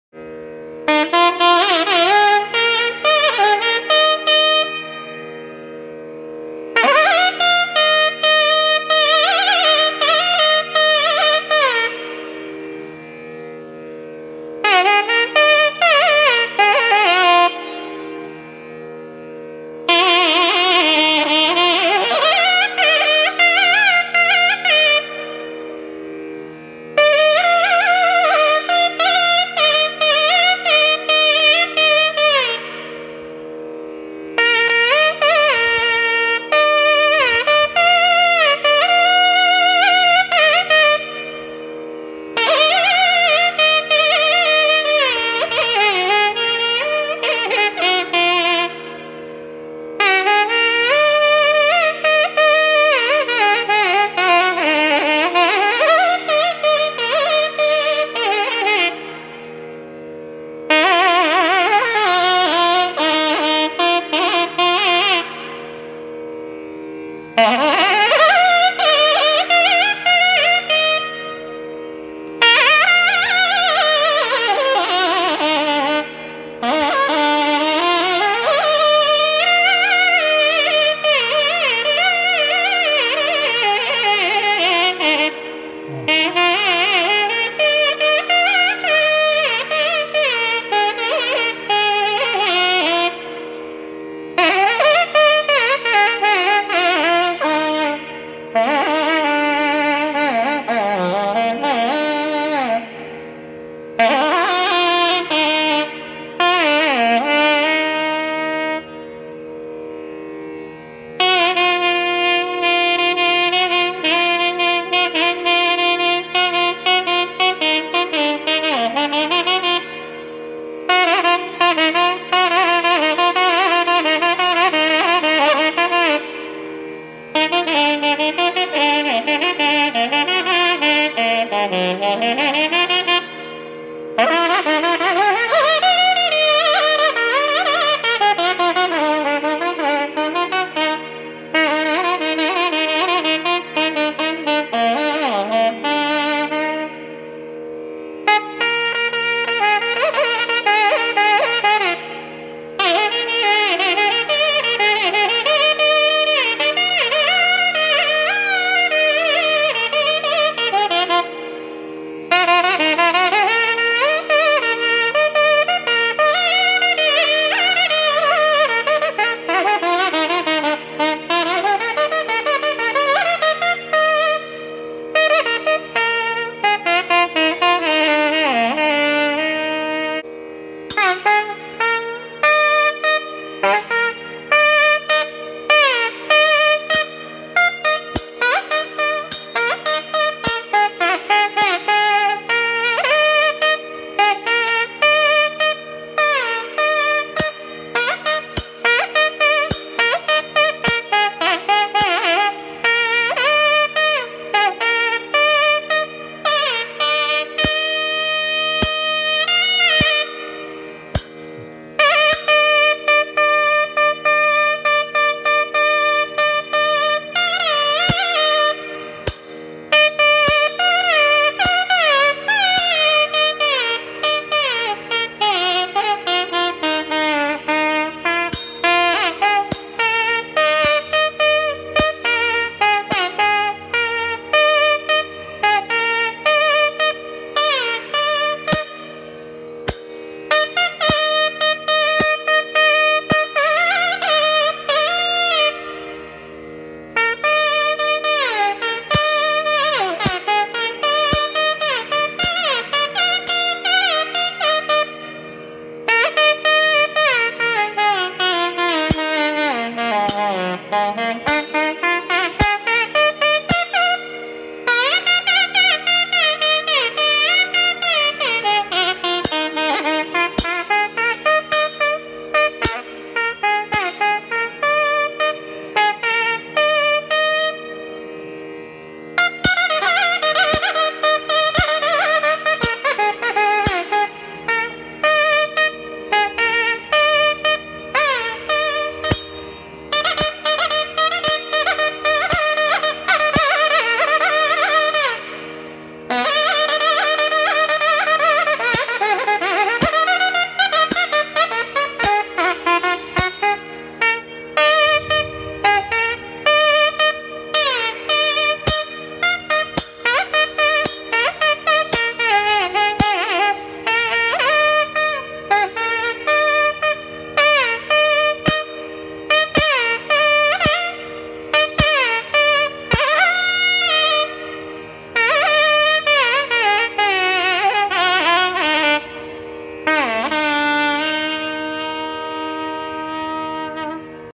Navaratri Navaraga Devata Ula - Rishipriya
On Day 8, we present another rare raga that has been rendered in concerts by Vid. Madurai Somu.

Vid. Injikudi Mariyappan renders a crisp ragam tanam and pallavi in this raga.